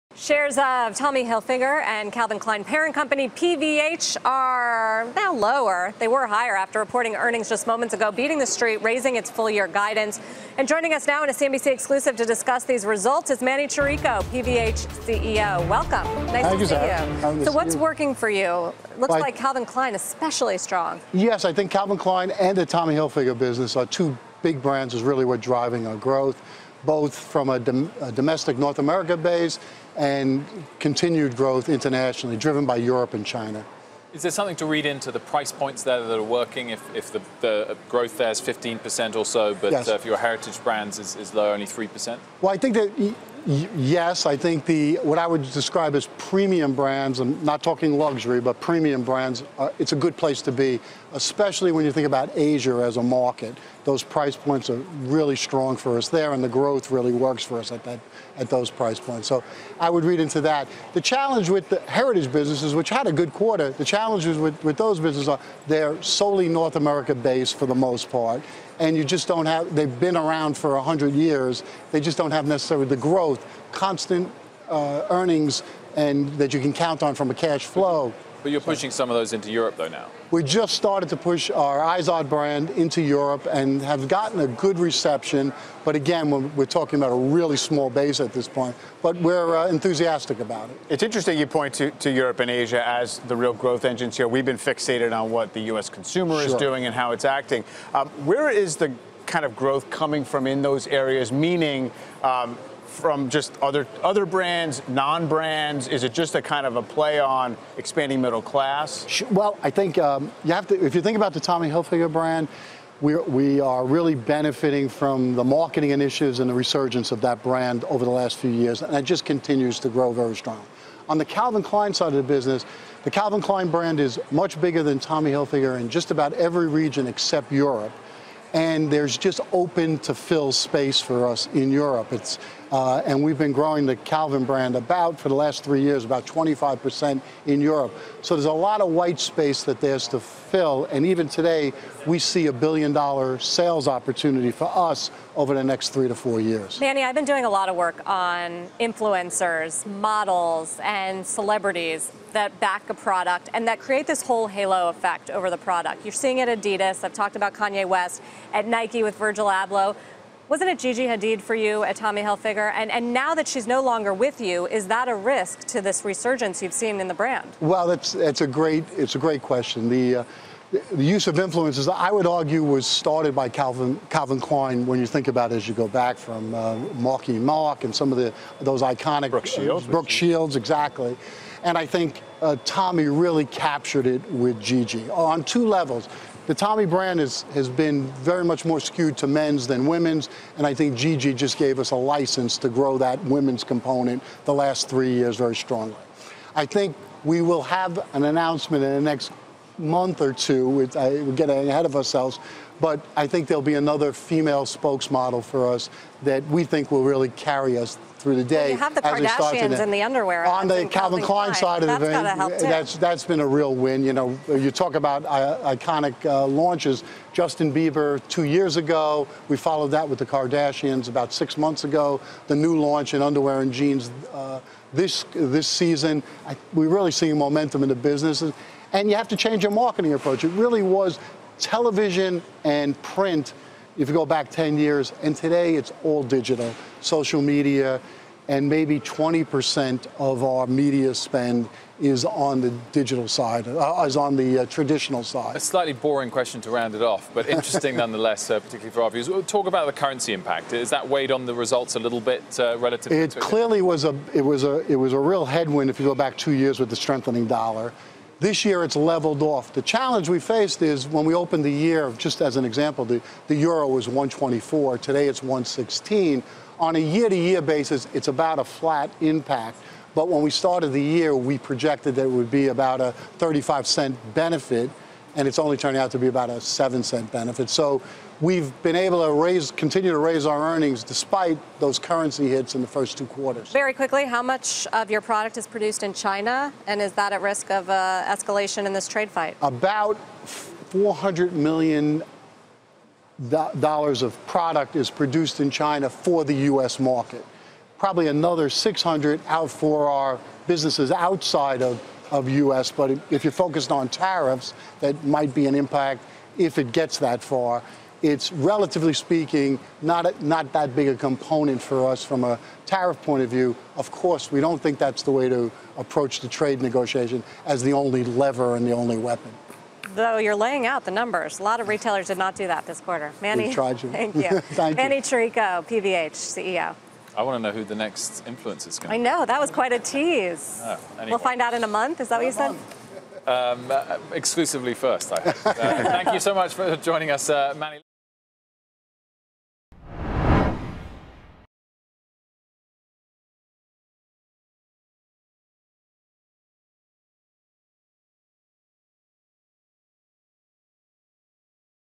PVH CEO Emanuel Chirico discusses quarterly earnings
PVH CEO Emanuel Chirico discusses the retailer's second quarter earnings report.